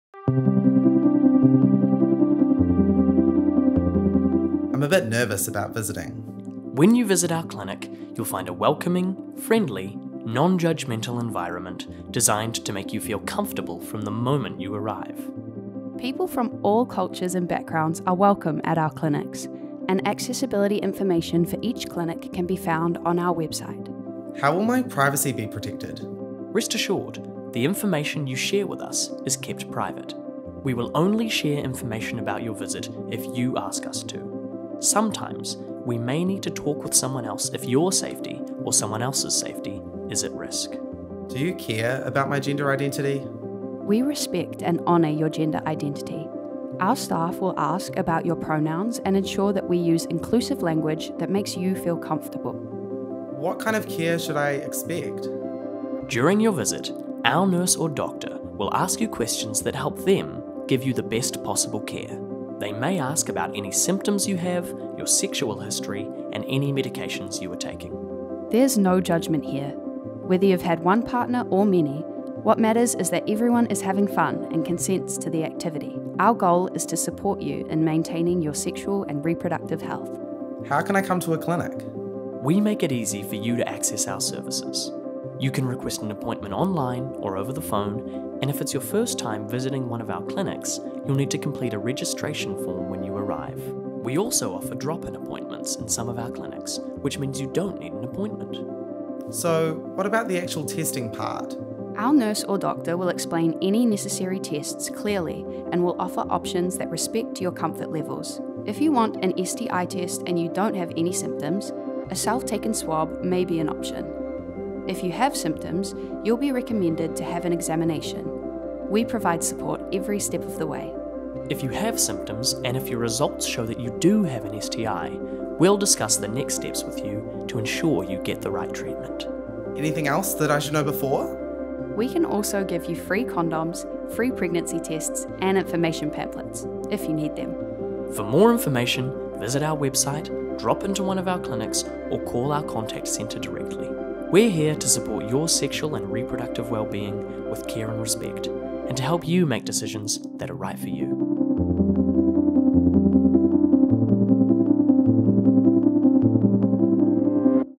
What to expect: visiting a clinic
Everything is easier when you know what to expect. Learn more about what it's like to visit one of our clinics